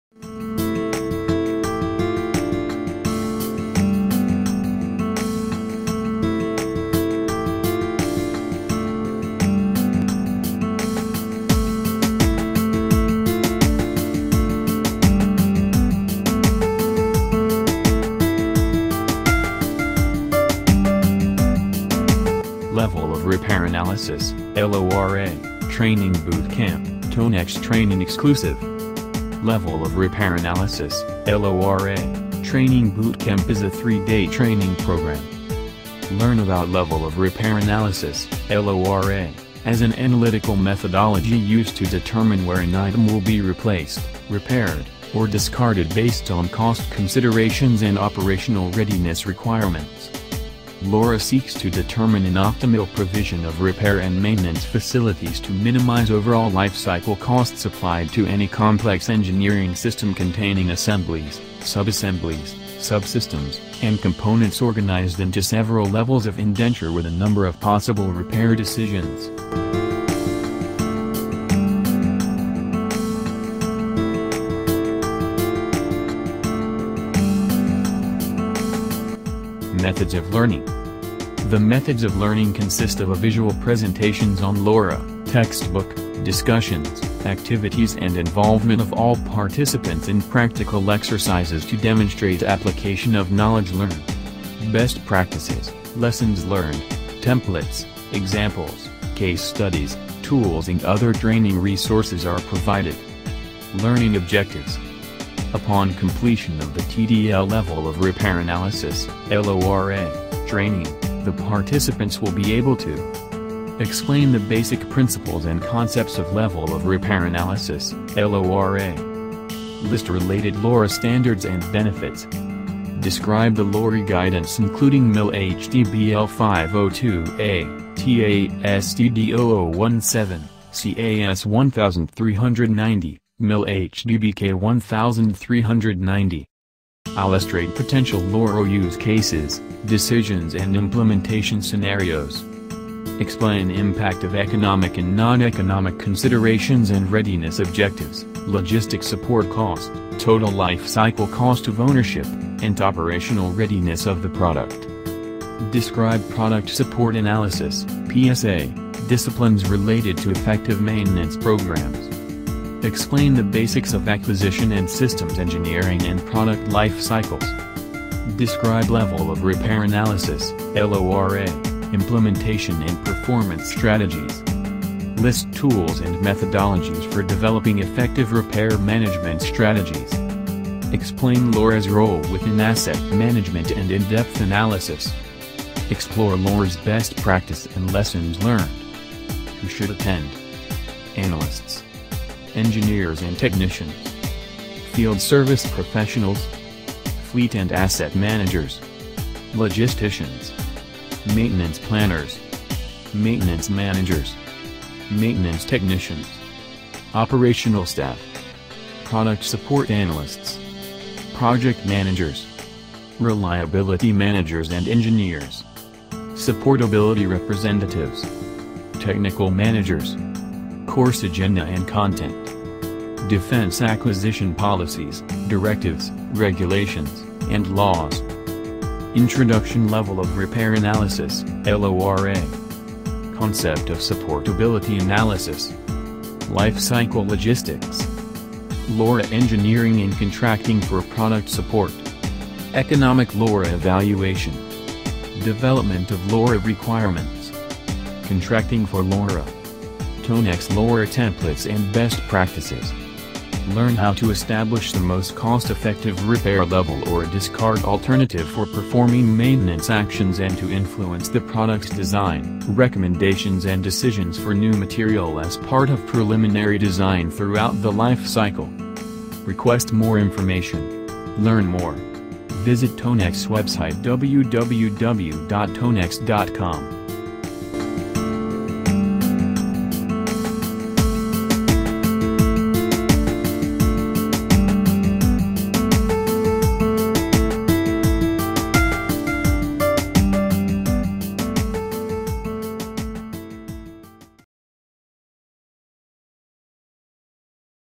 Level of Repair Analysis (LORA) Training Bootcamp : Tonex
Level of Repair Analysis (LORA) Training Bootcamp is a 3-day training program. Learn about Level of Repair Analysis (LORA) as an analytical methodology used to determine where an item will be replaced, repaired, or discarded based on cost considerations and operational readiness requirements.

LORA seeks to determine an optimal provision of repair and maintenance facilities to minimize overall life-cycle costs applied to any complex engineering system containing assemblies, sub-assemblies, subsystems, and components organized into several levels of indenture with a number of possible repair decisions.

Learning Objectives:

Upon completion of the TDL Level of Repair Analysis (LORA) training, the participants will be able to:

Explain the basic principles and concepts of Level of Repair Analysis (LORA)
List related LORA standards and benefits
Describe the guidance on LORA including MIL-HDBL-502A, TA-STD-0017, SAE AS1390 and MIL-HDBK-1390
Illustrate potential LORA use cases, decisions and implementation scenarios
Explain impact of economic and noneconomic considerations and readiness objectives, logistic support cost, total life cycle cost of ownership, and operational readiness of the product
Describe Product Support Analysis (PSA) disciplines related to effective maintenance programs
Explain the basics of acquisition and systems engineering and product life cycles
Describe Level of Repair Analysis (LORA) implementation and performance strategies
List tools and methodologies for developing effective repair management strategies
Explain LORA’s role within asset management and in-depth analysis
Explore LORA’s best practice and lessons learned

Who Should Attend:

Analysts
Engineers and technicians
Field service professionals
Fleet and asset managers
Logisticians
Maintenance planners
Maintenance managers
Maintenance technicians
Operational staff
Product support analysts
Project managers
Reliability managers and engineers
Supportability representatives
Technical managers

Course Agenda and Content:

Defense Acquisition Policies, Directives, Regulations, and Laws
Introduction Level of Repair Analysis (LORA)
Concept of Supportability Analysis
Life Cycle Logistics
LORA Engineering and Contracting for Product Support
Economic LORA Evaluation
Development of LORA Requirements
Contracting for LORA
TONEX  LORA Templates and Best Practices

Learn how to establish the most cost effective repair level or discard alternative for performing maintenance actions and to influence the product’s design, recommendations and decisions for new material as part of preliminary design throughout the life cycle.